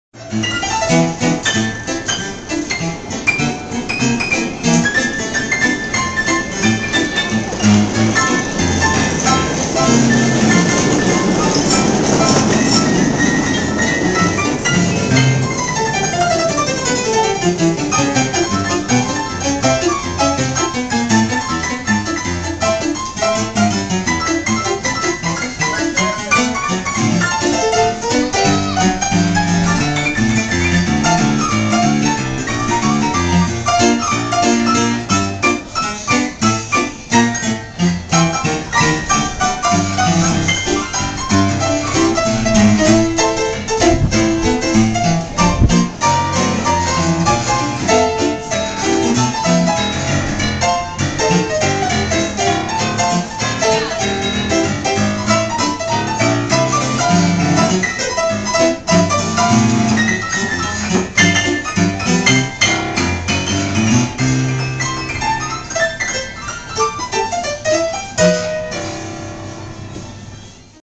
Piano Player
On Main Street in the Magic Kingdom, July 2010